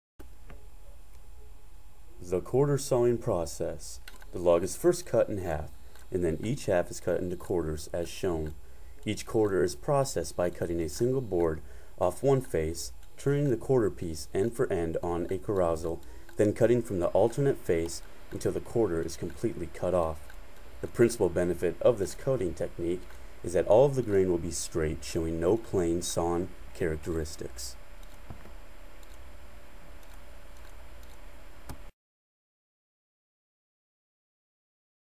The Quatersawing Process
How to quartersaw a piece of oak wood to create a beautiful grain design in your next wood working project.